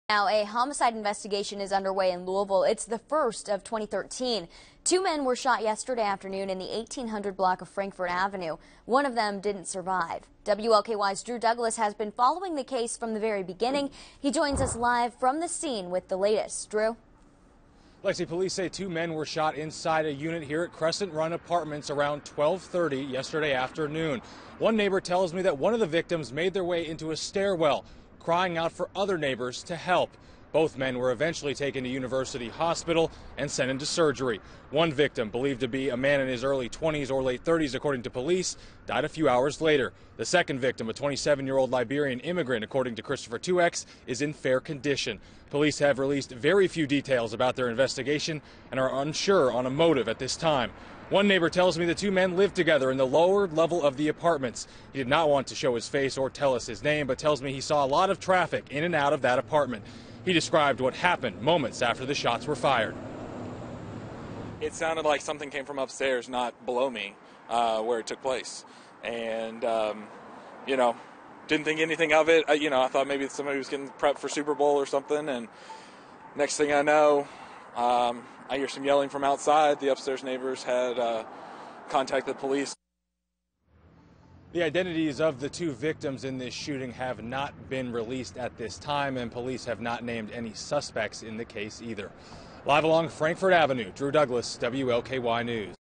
Frankfort Ave. shooting leaves 1 dead, 1 injured
Louisville metro police homicide unit is investigating after a person shot on Frankfort Avenue died.